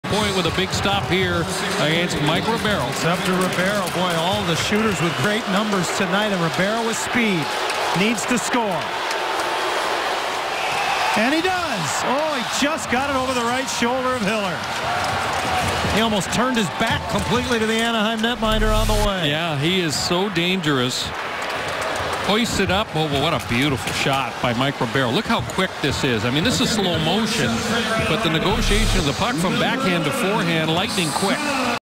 Mike Ribeiro's amazing shootout move
Phoenix Coyote forward Mike Ribeiro makes a great move to score in the shootout.